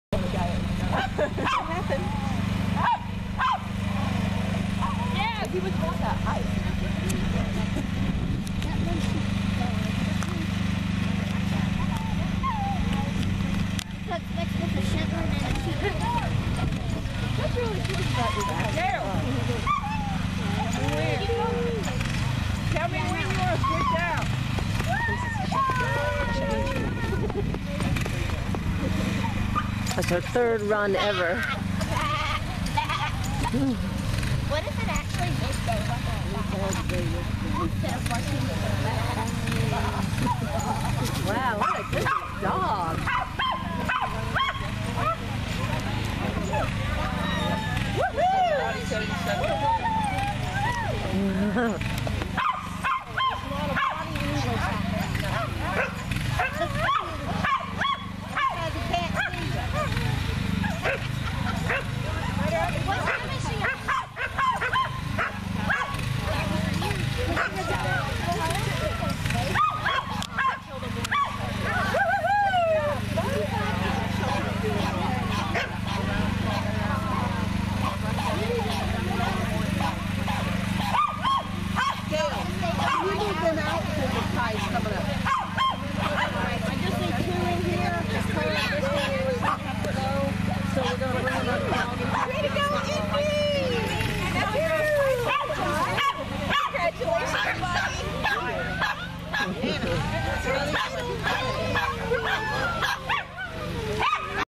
Indi the Sheltie's Third Coursing Ability Test (CAT)
And that's title, my friends =)  Yesterday, I wasn't sure he would even chase the lure.  This mornin he was raring to go and straining at the leash on the start line.

Thanks to the Doberman Pinscher Club of Charlotte for organizing, Brylin Obedience Specialty School for hosting and Traci Niederritter for judging. We had a great time this weekend and can't wait to come back next time!